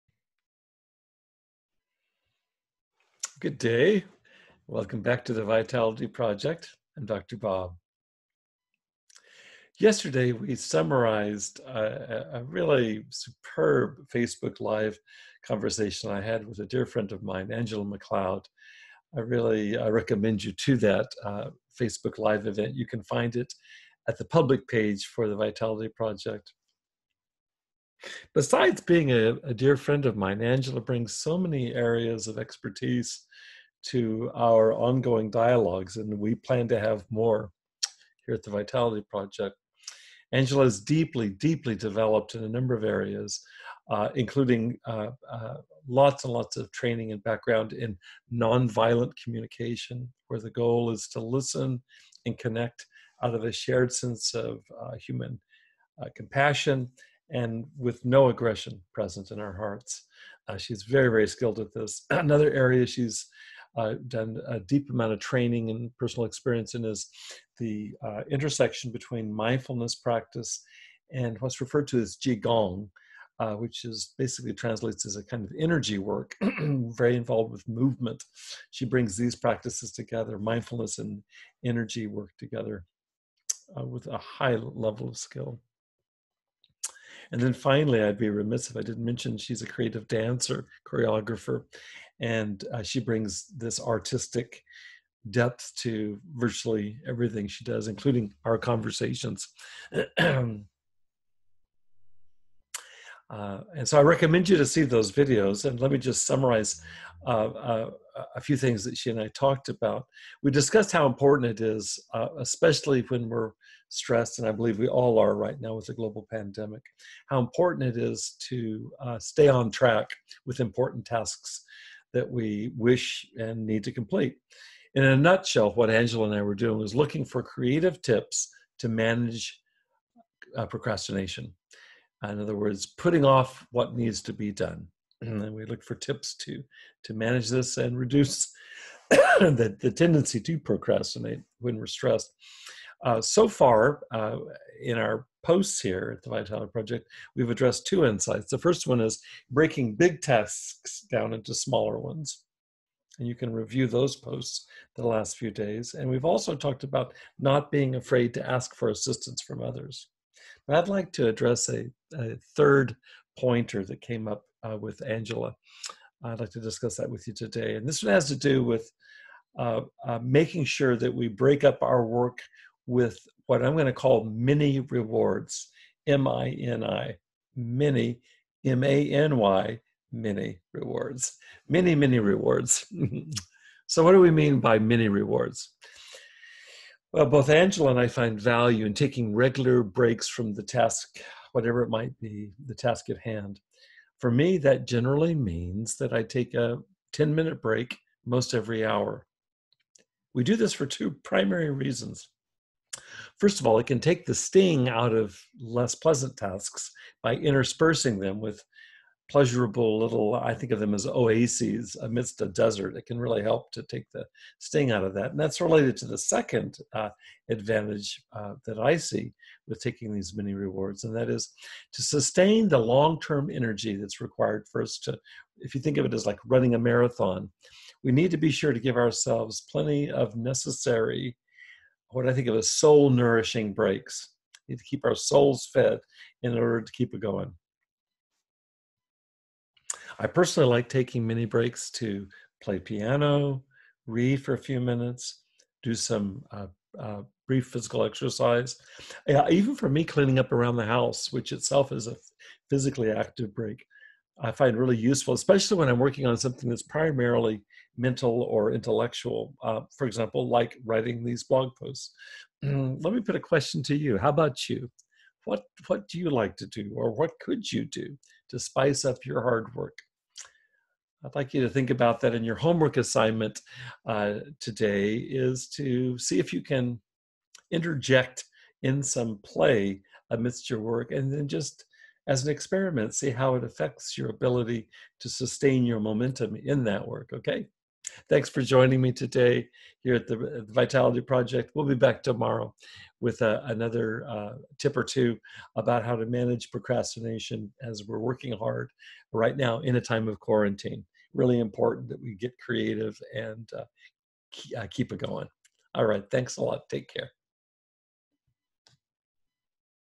How to Reward Ourselves Amidst Completing Tasks | 4/9/20 | Rollin' Out of Bed With Dr. Bob
Good day, and welcome back to the Vitality Project. I'm Dr. Bob!

Yesterday we summarized this week’s excellent Facebook Live conversation with dear Angela McLeod. Besides being a dear friend, Angela brings so many areas of expertise to our ongoing dialogues here at the Vitality Project. She is deeply, deeply developed in: 1) helpful and practical approaches to non-violent communication (listening and connecting out of a shared sense of human compassion, with no aggression present in our hearts); 2) mindfulness and qi gong (energy work) practices; and 3) creative dance – just to name three domains that are radically apparent as you interact with Angela.

Angela and I discussed how important it is, especially when we are stressed (like now, with the global pandemic), to stay on-track with important tasks we wish to complete. In a nutshell, we were looking for creative tips to manage procrastination, that is, putting off what needs to be done.

We addressed two insights: 1) breaking down big tasks into smaller ones; and 2) not being afraid to ask for assistance from others. I’d like to discuss a third pointer today. This one has to do with making sure to break up our work with “mini-rewards.” Now what do Angela and I mean by that?

We both find value in taking regular breaks from the task at hand – for me, that generally means I take a 10-minute break most every hour. We do this for two reasons: 1) it can take the sting out of less pleasant tasks by interspersing them with pleasurable little “oases”; and 2) to sustain the long-term energy to “run a marathon,” we need to be sure to give ourselves necessary, soul-nourishing breaks in order to “keep a’goin’.”

I like taking mini-breaks to play piano, or read, or do some physical exercise – even clean up around the house (which itself provides a physically active break) – especially when I’m working on something primarily mental or intellectual (like writing blog posts!).

How about you? What can you do to spice up your hard work?